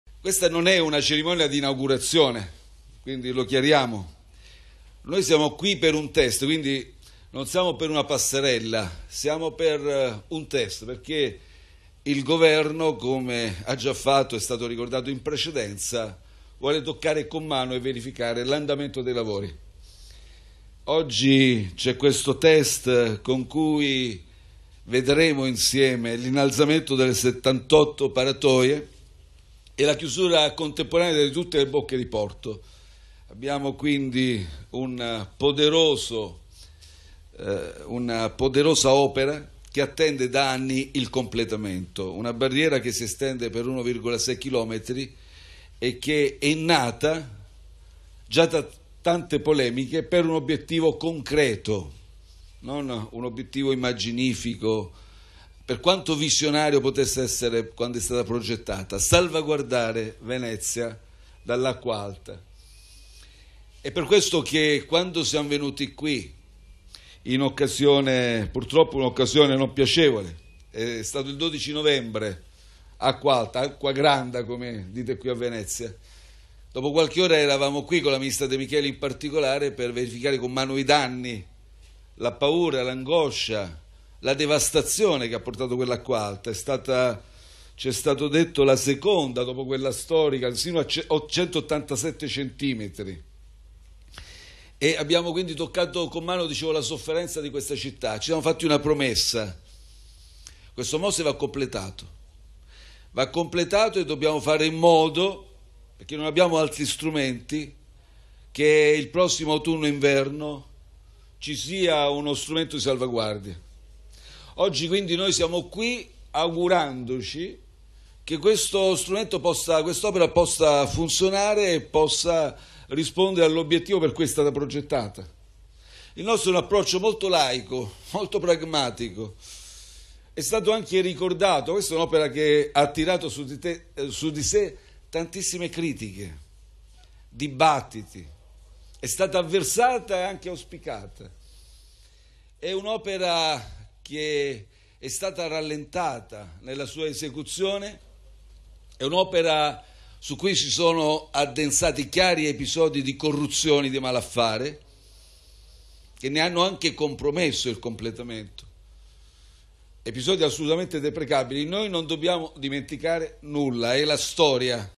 Mose, Conte a Venezia: «Nessuna cerimonia, non è una passerella ma un test»
Il premier con una nutrita delegazione del governo per assistere al sollevamento delle paratoie

\"Sono qui con alcuini esponenti del governo per un test, non per una passerella. Questa non è una cerimonia\". Il premier Conte ci tiene a precisarlo in avvio di conferenza stampa a Venezia, dove si è recato con le ministre De Micheli e Lamorgese e il ministro D'Incà per assistere al sollevamento delle paratoie di contenimento del Mose. ( Ansa - CorriereTv ). Guarda il video su Corriere